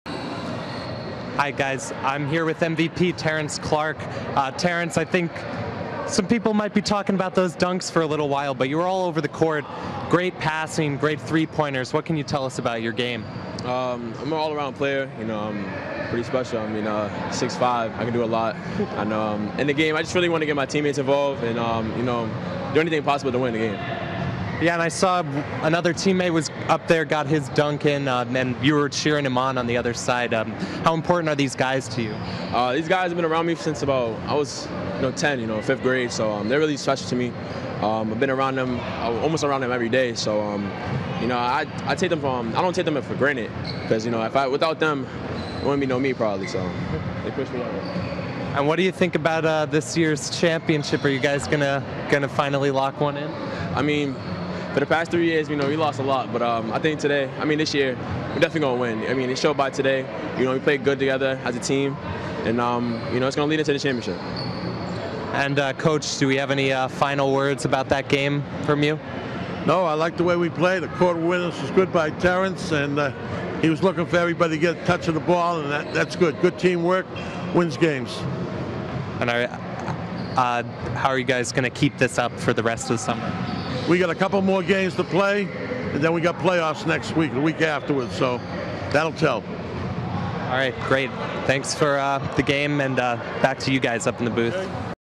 Game of the Week MVP Interview: Titans' Terrence Clarke & Assistant Coach Al Brodsky
Game of the Week sideline reporter Donovan Burton interviews MVP Terrence Clarke (20 pts, 12 rebs, 7 asts.) and assistant coach Al Brodsky, after the Titans defeated the Lee School, 66-57, in the Boys BNBL 15 & Under division at the Perkins Community Center in Dorchester.   To view the Titans vs. Lee School Boys BNBL 15 & Under basketball game in its entirety, please